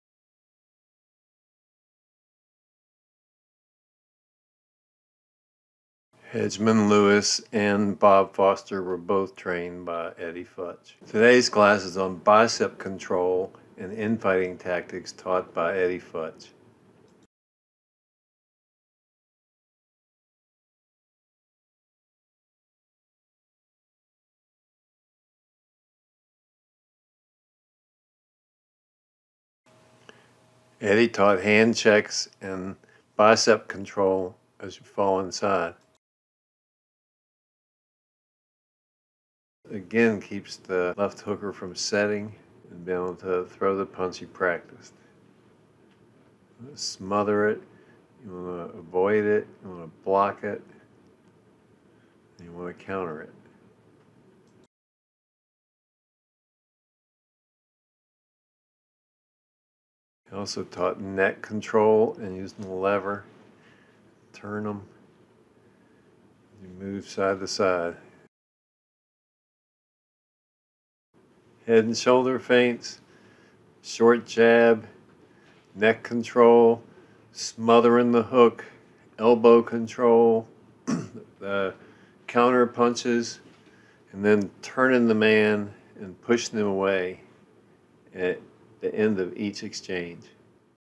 Boxing with Wilson Pitts - bicep control
infighting taught by Eddie Futch
Bob Foster Hedgemon Lewis